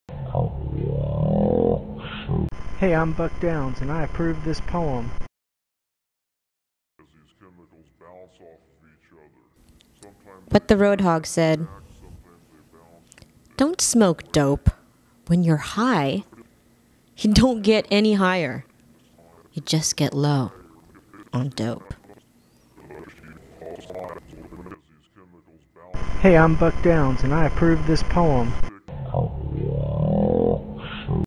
Buck Downs Poetry - "What The Road Hog Said" - 23 Aug 2001
My friend Buck Downs has been sending me 1 poem a month in the mail for 10 years.  In return I am having people read them on video.